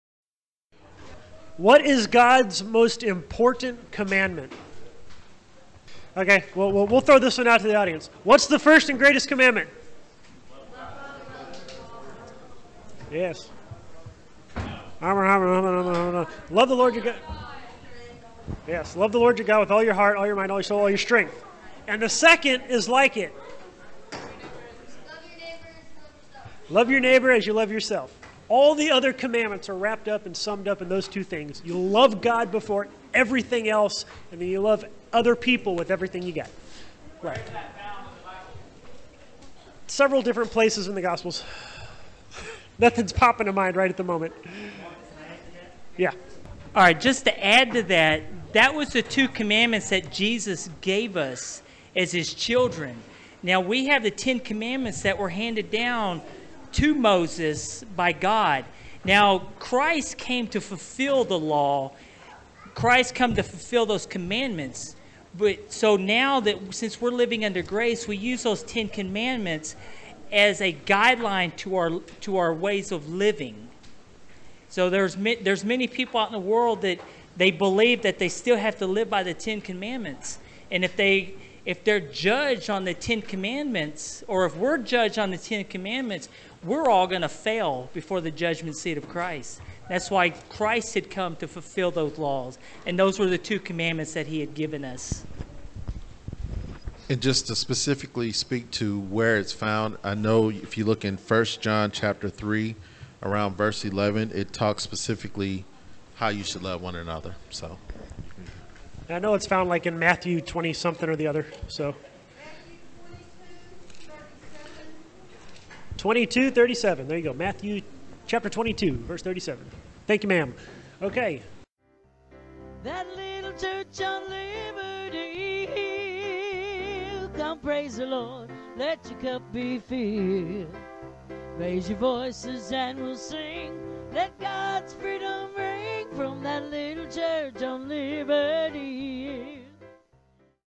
What is the greatest commandment? (October 2012 Q&A)
2nd Q&A Event with the Youth Group at The Church at Tree Lake in October 2012 with Rodrick Kennerson-Baty, David Dover, Jeff Bradburn, Carl Dolifka, & Marc Lambert.